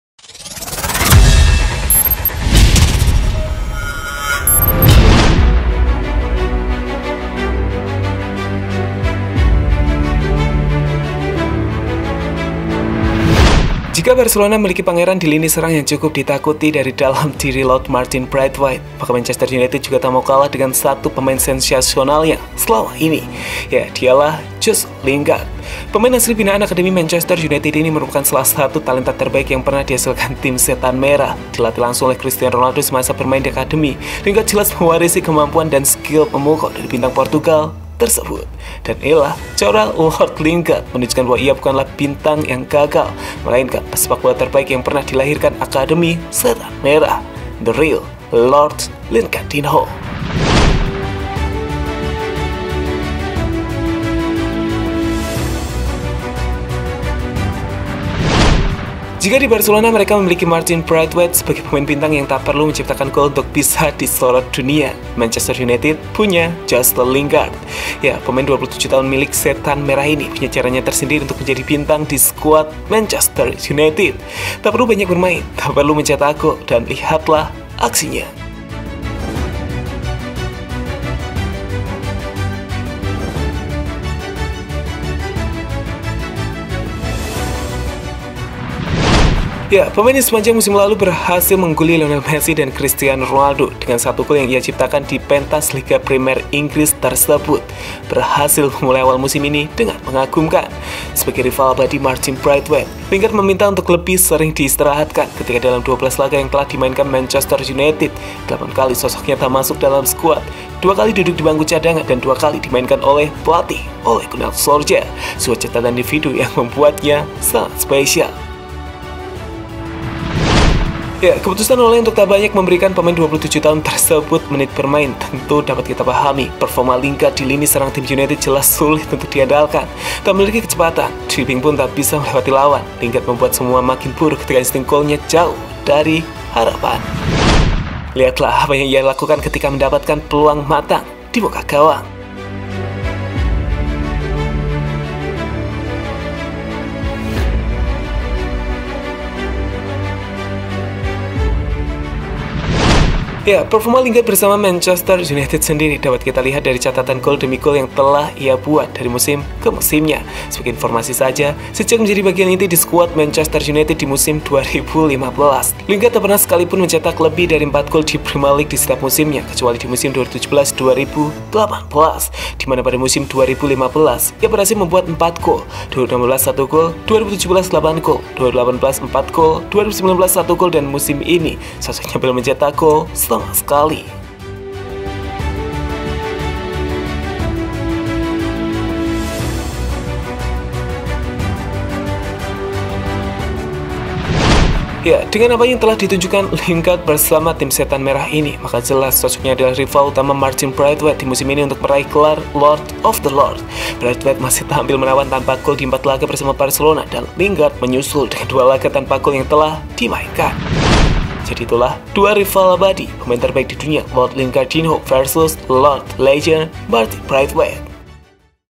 Rival Sesungguhnya Lord Braithwaite !!! Lihat Performa Lingard Dalam Membuktikan Jadi Bintang MU
Garuda Legend Team Official :

Editor video :@Alvansabil
Pengisi Suara ;@Topik36
Admin :@Arihidayat22

GL News
GALZ
Garuda Legend
Timnas Legend